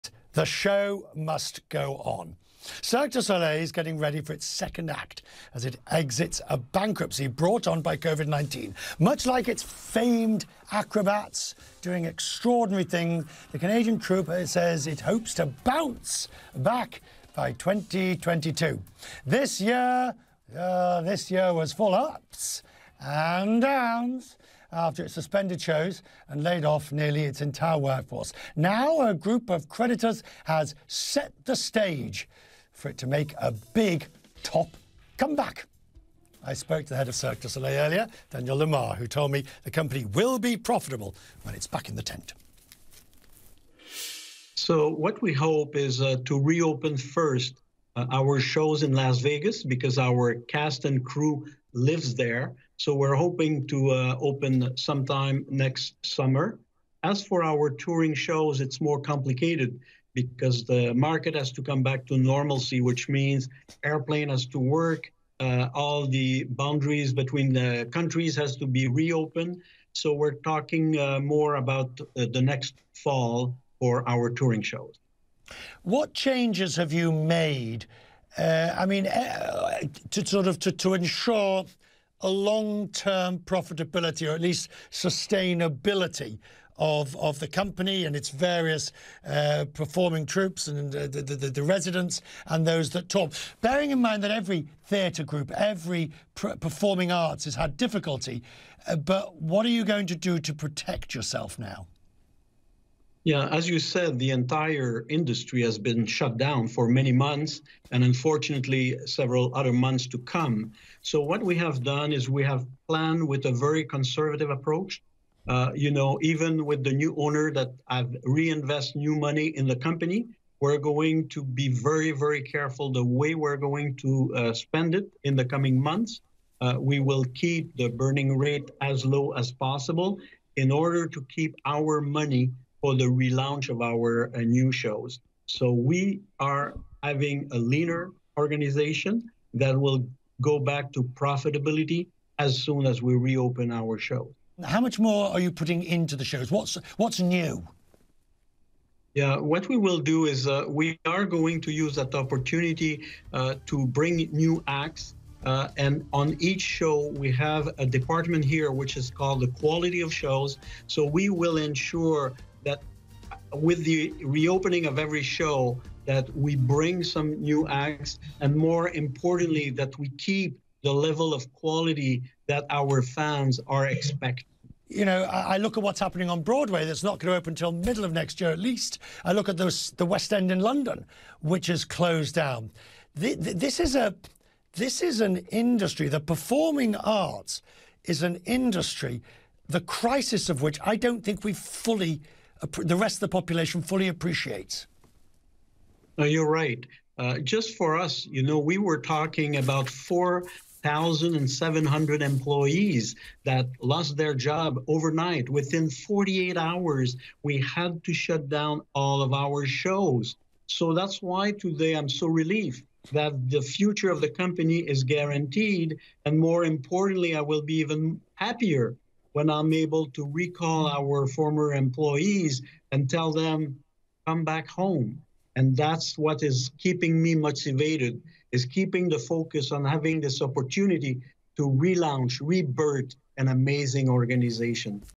Cirque du Soleil CEO: We will return to profitability
Cirque du Soleil is bouncing back after bankruptcy — and getting ready for a big (top) comeback, complete with new acts. "That's what is keeping me motivated," CEO Daniel Lamarre says. "Keeping the focus on having this opportunity to relaunch, rebirth an amazing organization."